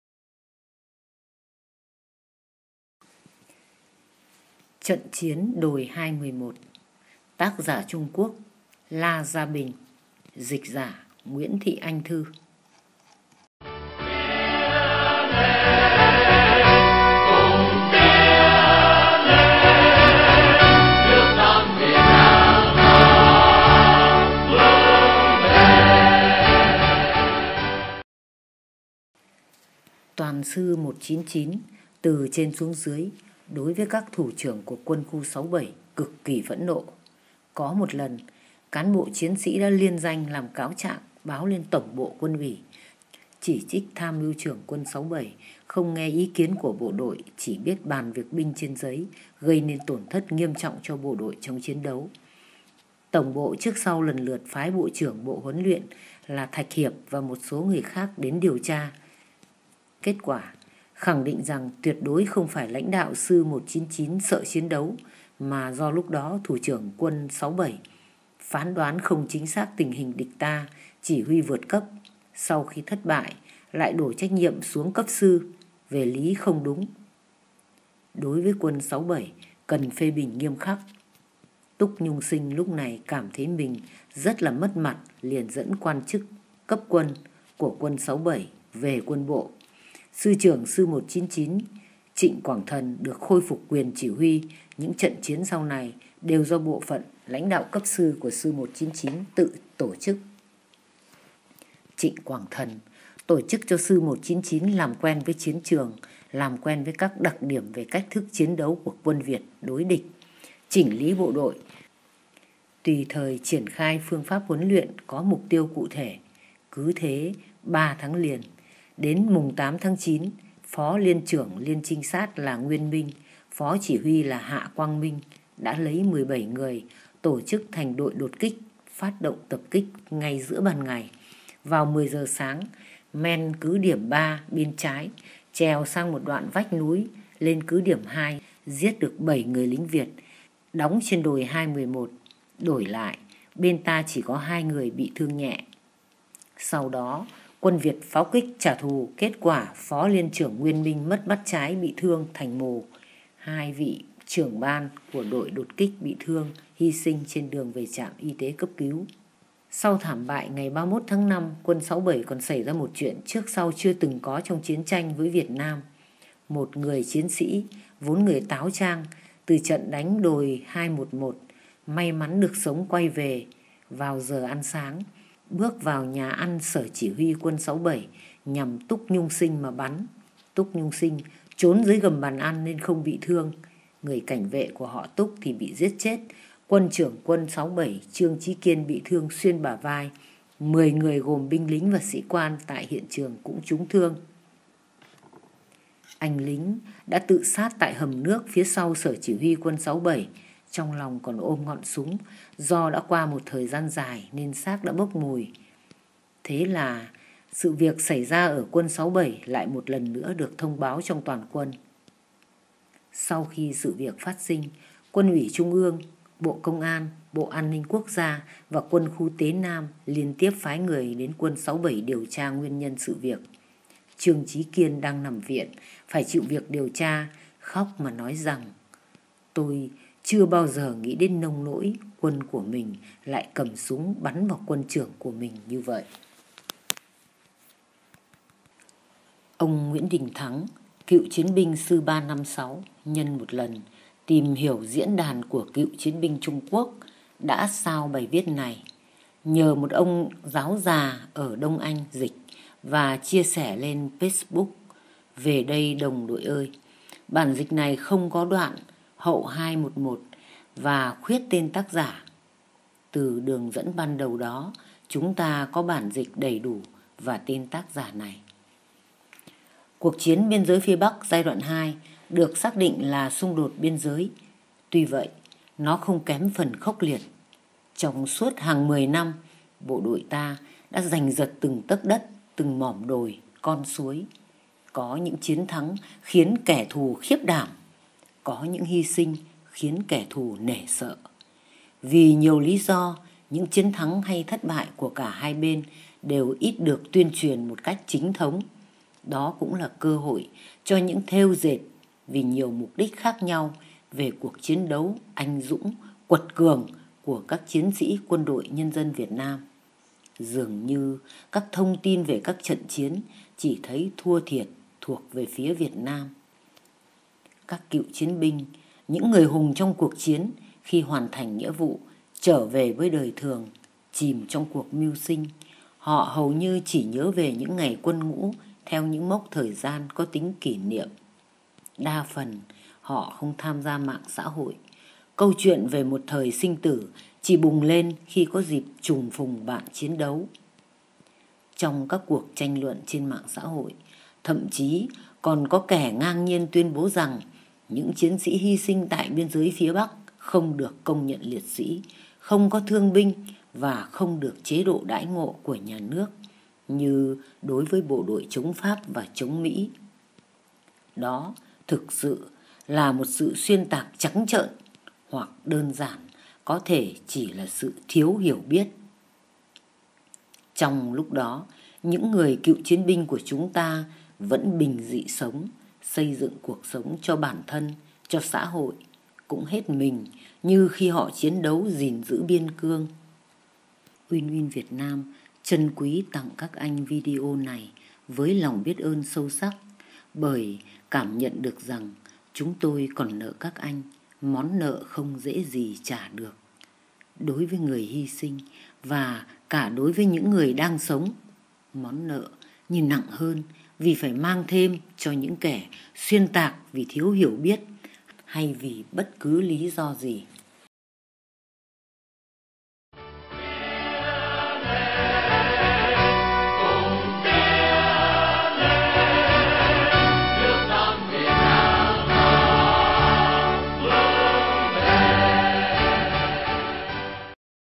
Trận chiến đồi 211 (tiếp theo và hết)
Win win Việt Nam trân quý tặng các anh video này như lòng biết ơn sâu sắc, bởi cảm nhận được chúng tôi còn nợ các anh, món nợ không dễ gì trả được. Đối với người hi sinh và cả đối với những người đang sống. Món nợ như nặng hơn vì phải mang thêm cho những kẻ xuyên tạc vì thiếu hiểu biết hay vì bất cứ lý do gì.